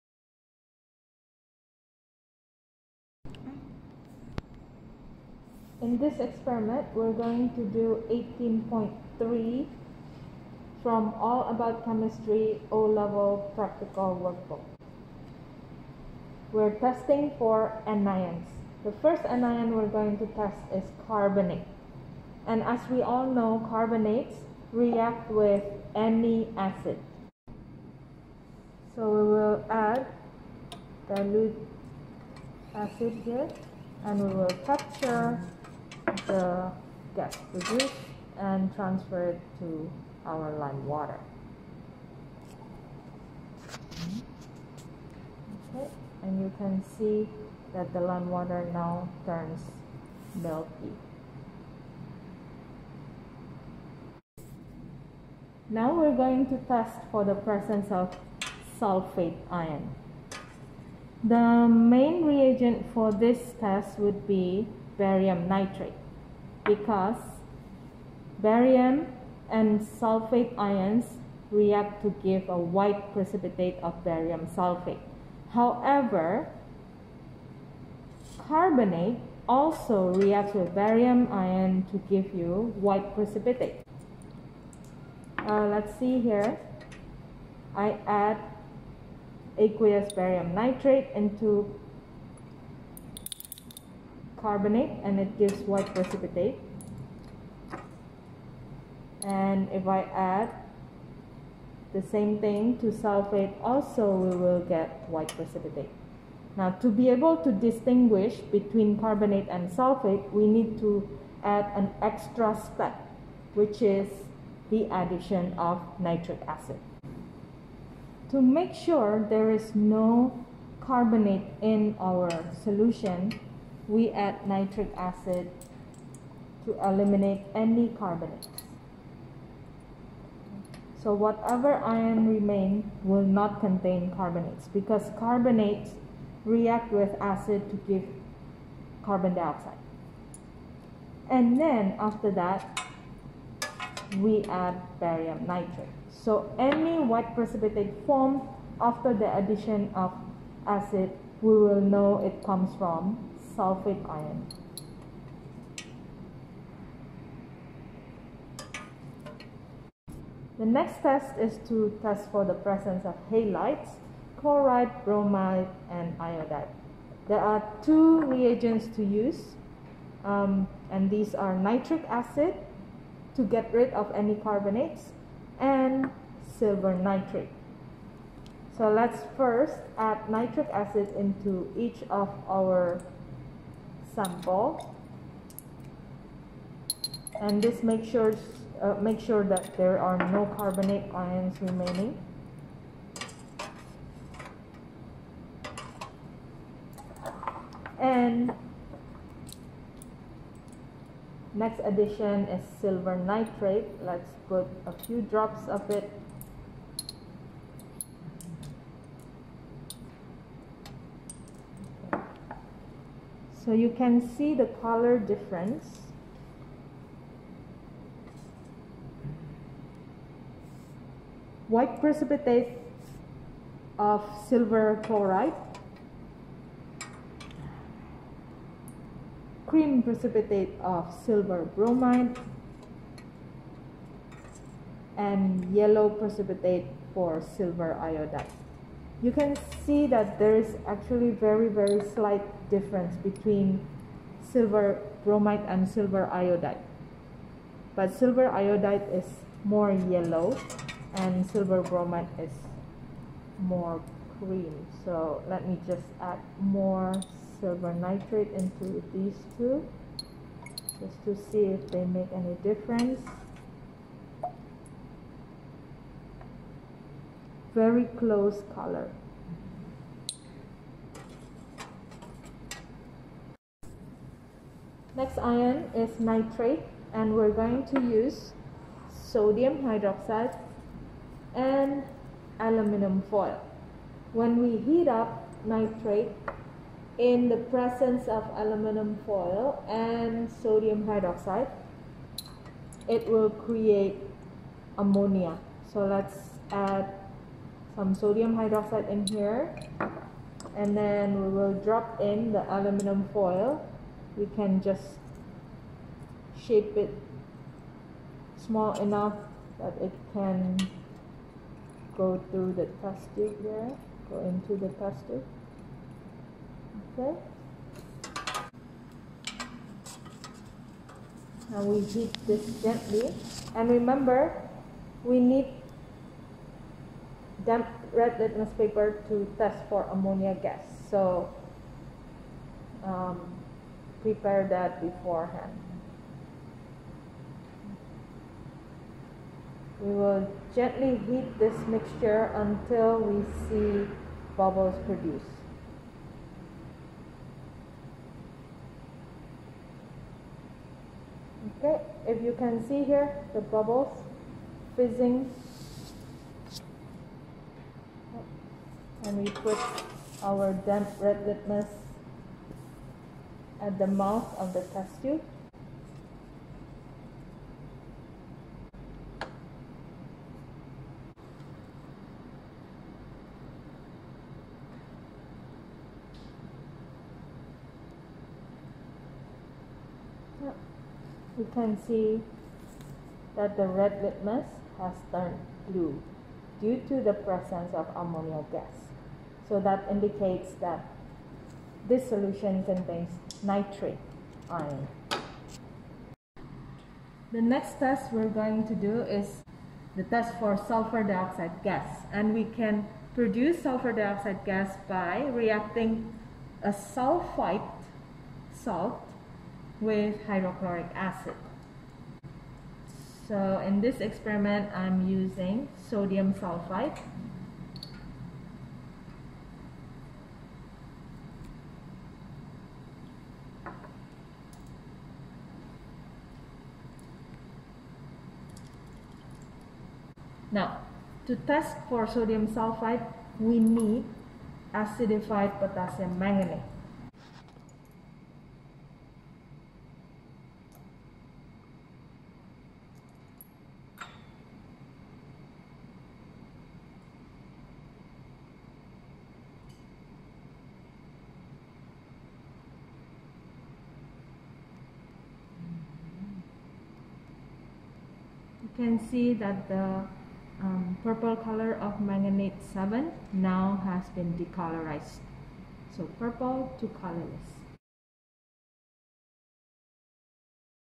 Tests for anions for IGCSE and O Level Chemistry
In this video, I am going to show you how to perform chemical tests for the anions carbonate, chloride, bromide, iodide, nitrate, sulfate, and sulfite. These anion tests are required in IGCSE and O Level Chemistry syllabi.

0:00 Source, carbonate
0:55 Sulfate
2:40 Halides
4:34 Nitrate
6:36 Sulfite